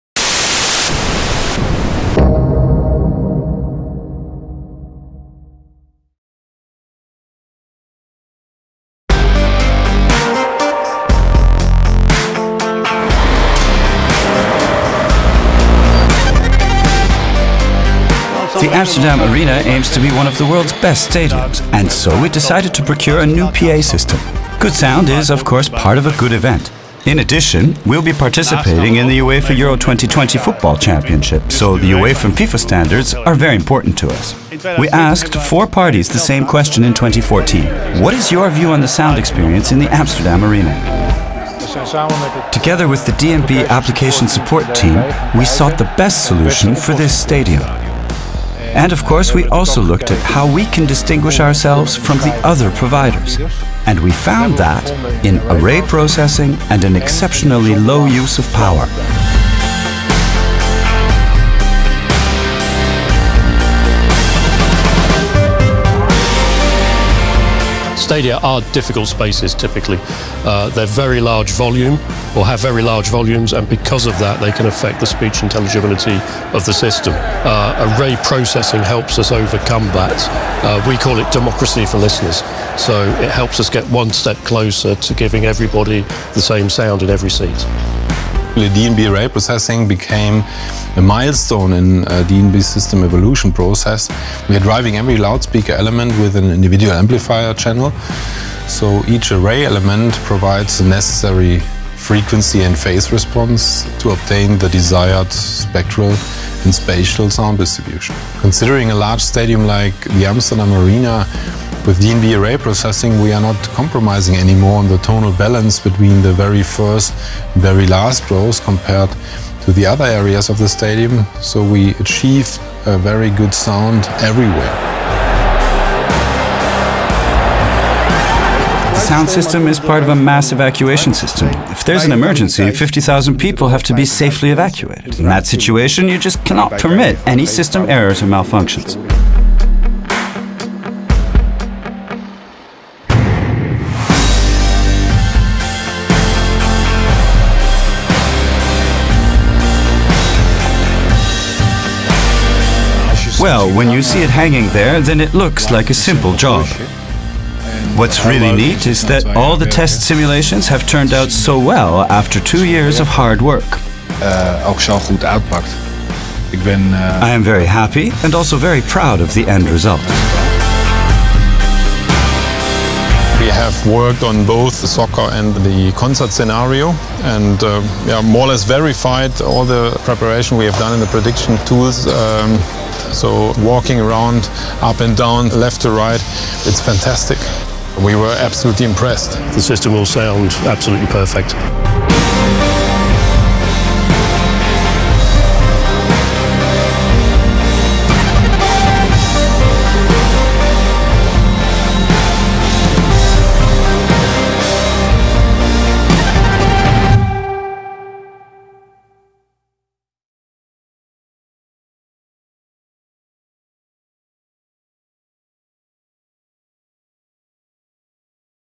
Amsterdam ArenA with ArrayProcessing. d&b On location (EN)
Amsterdam ArenA, Netherlands

For Amsterdam ArenA, the drive to be one of the world’s best stadiums meant investing in sound. Discover how the first venue of its kind to install a fully ArrayProcessed d&b system not only realized UEFA/FIFA regulations for performance and intelligibility, and national and international safety standards for evacuation systems, but did so with an easily reconfigurable solution, and equal, balanced sound from the first seat to the last. All the while, reducing the venue’s audio power consumption by 85%.